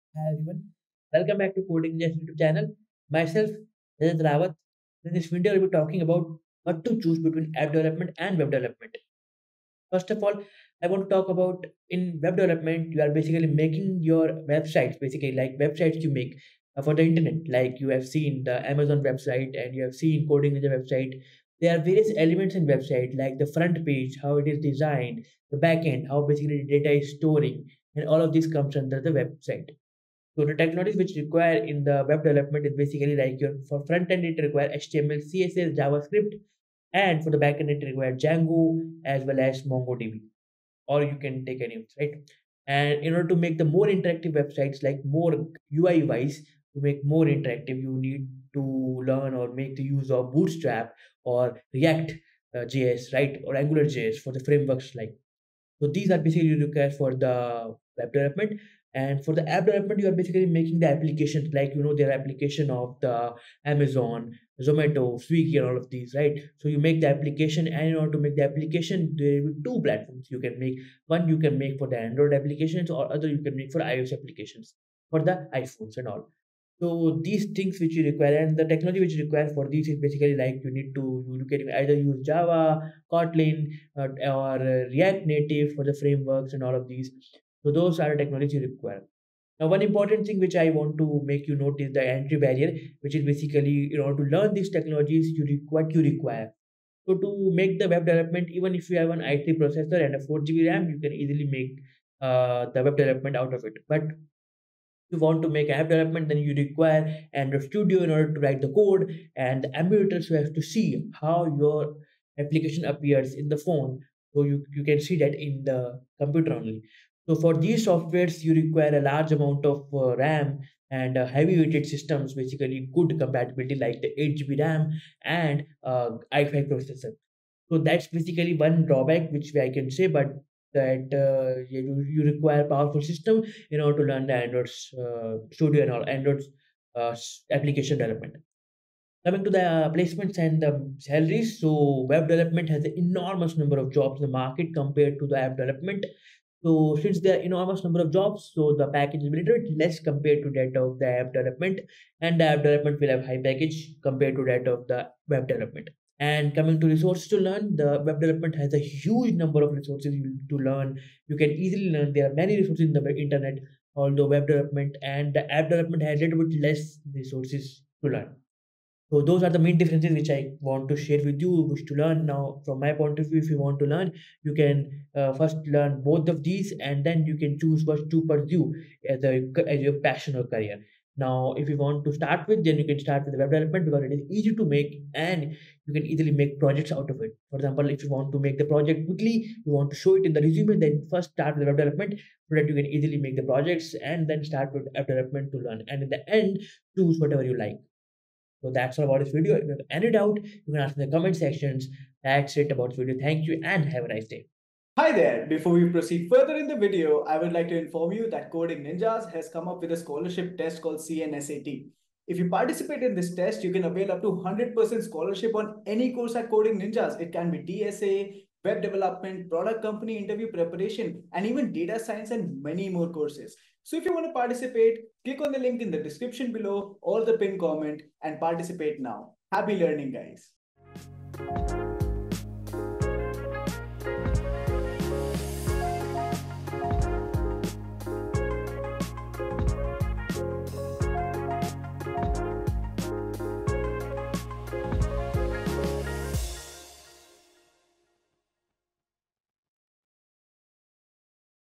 Web Development vs App Development | Which To Choose? | Best Way To Choose It | Coding Ninjas
BEST FREE Resource To Learn DSA, C++, Python, Java, Web Development, Competitive Programming,

In this video, we will discuss Web Development vs. App Development. Which one to choose?

Top COMPANIES To Apply For LATERAL ENTRY In INDIA 2022 |  @Coding Ninjas